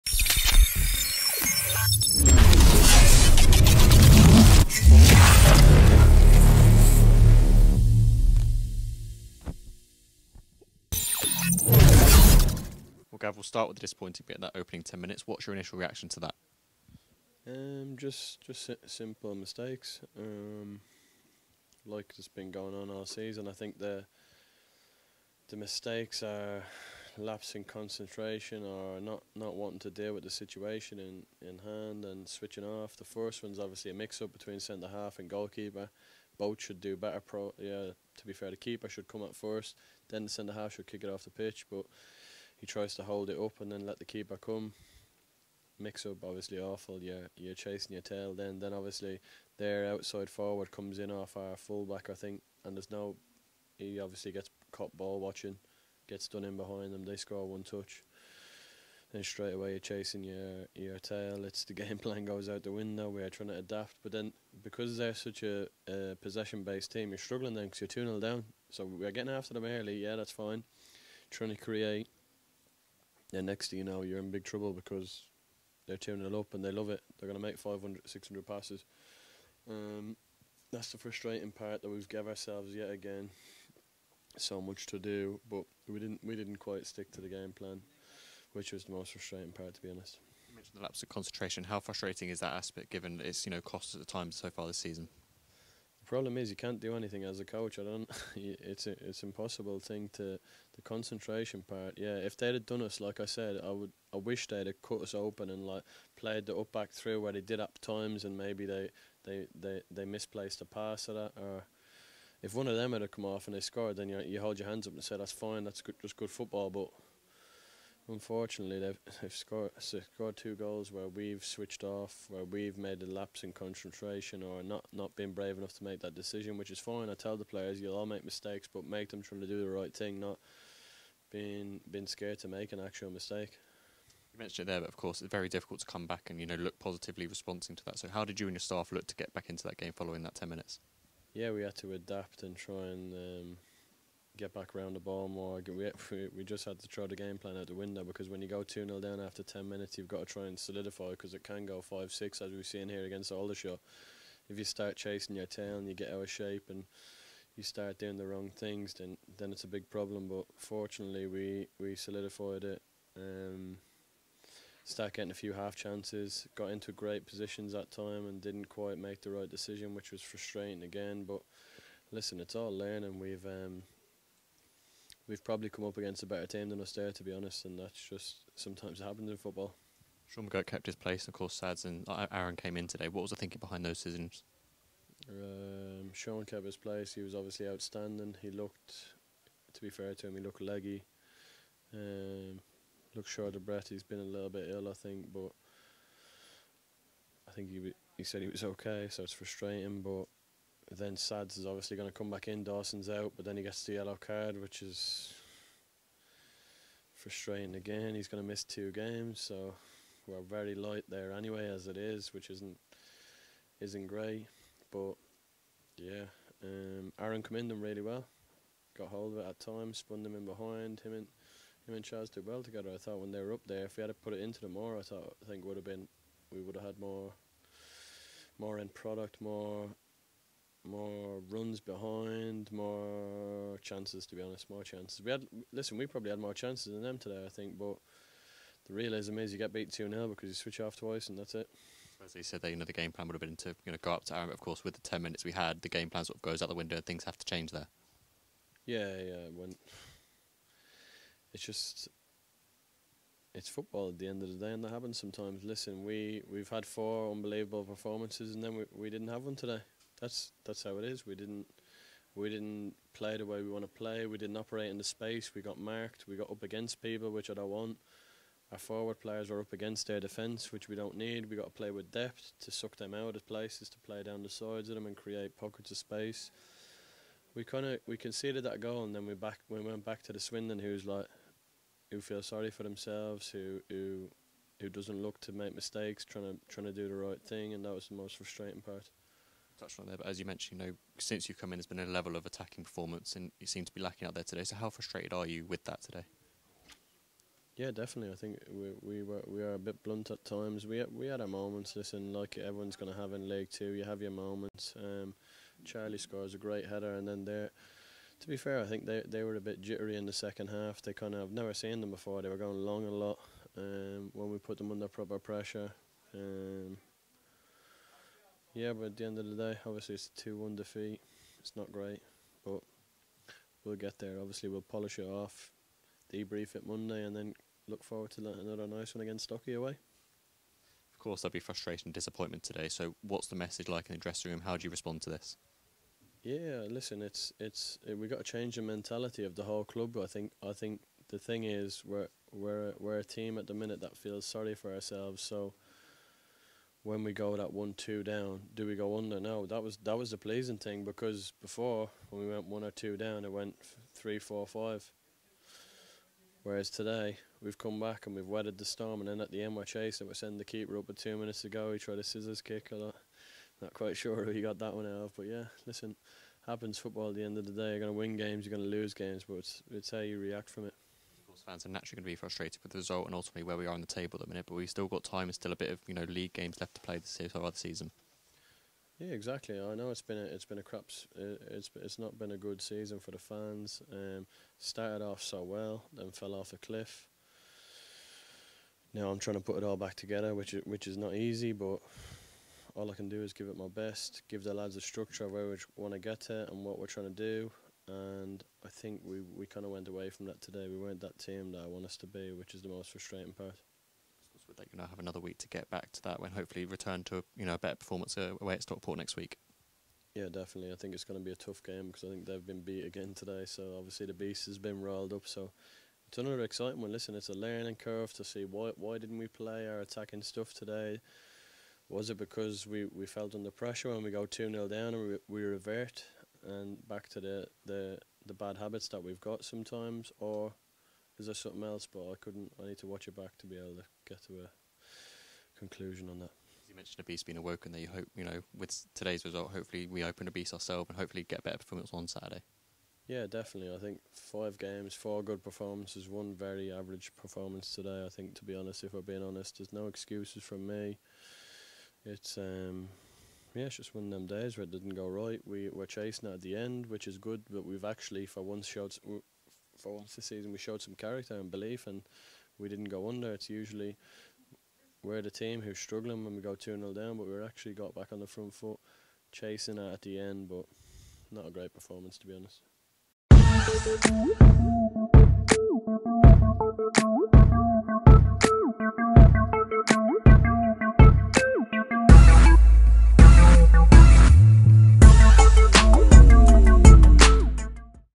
Gavin Gunning on defeat to MK Dons | Swindon Town Football Club
Interim Head Coach, Gavin Gunning on defeat against MK Dons at The County Ground.